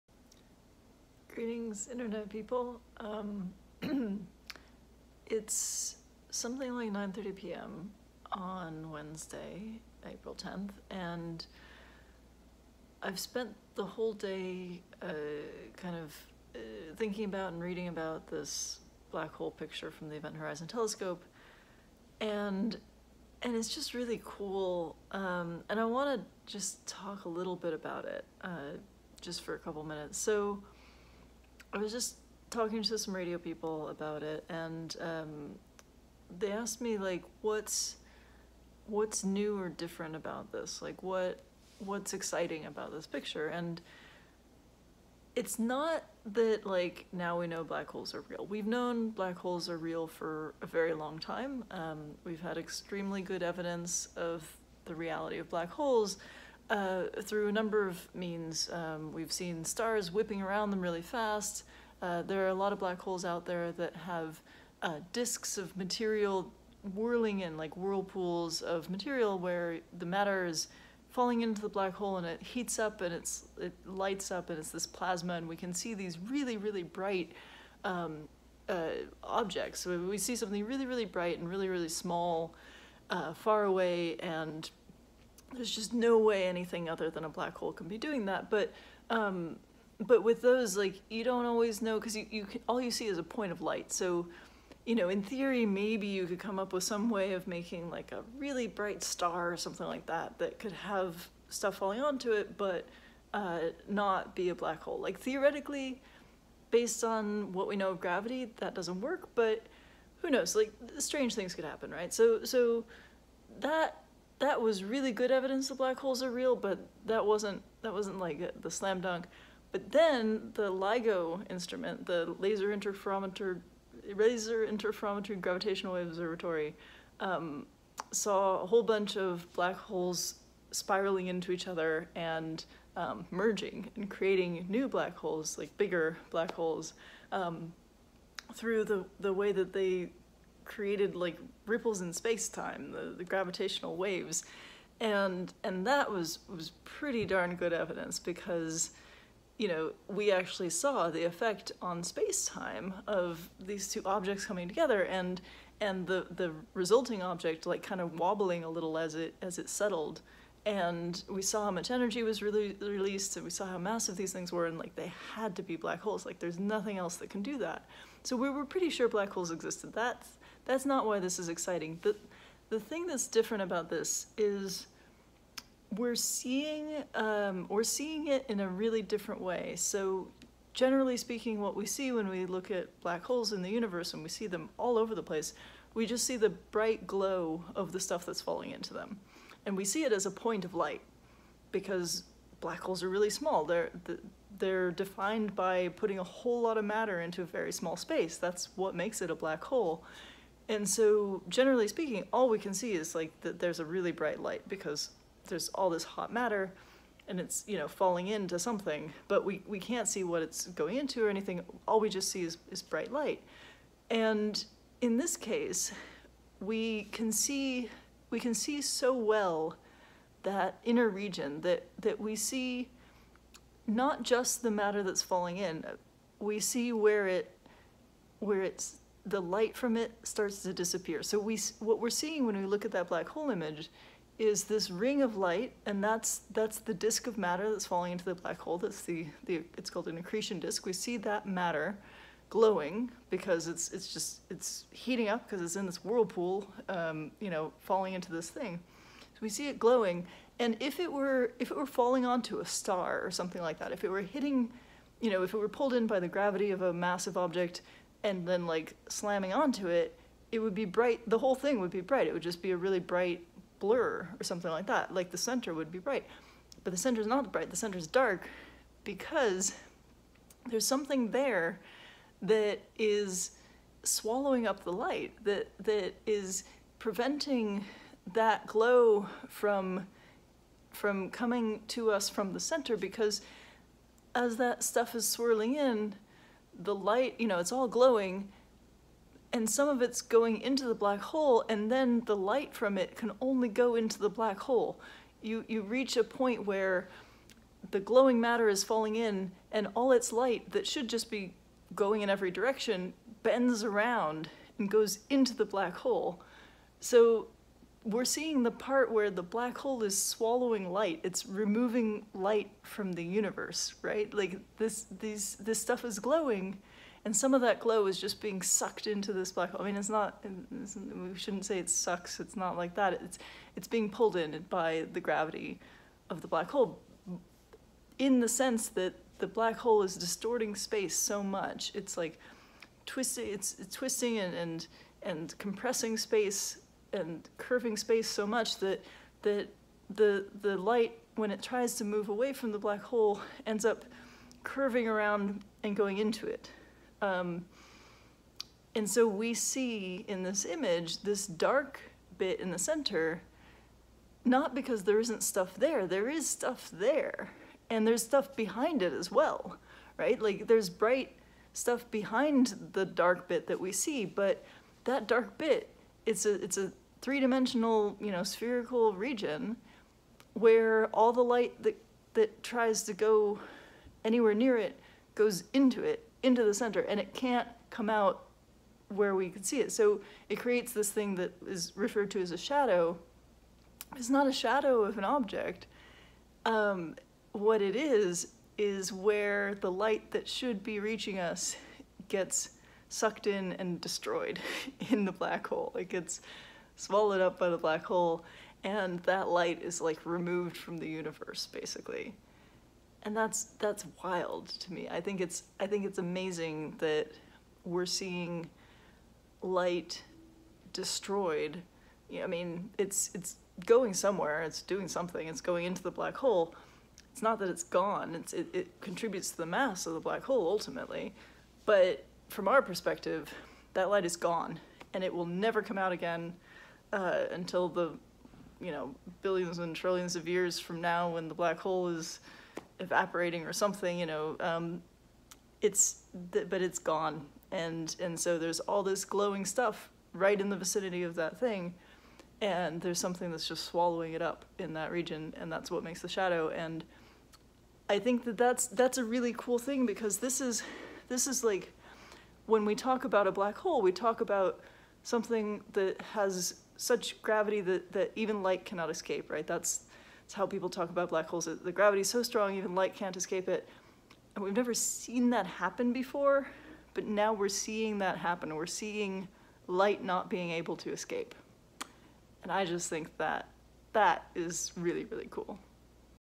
Rambling Enthusiasm about the Black Hole Picture
In which I enthuse about why the Event Horizon Telescope’s first image of a black hole is so ridiculously cool.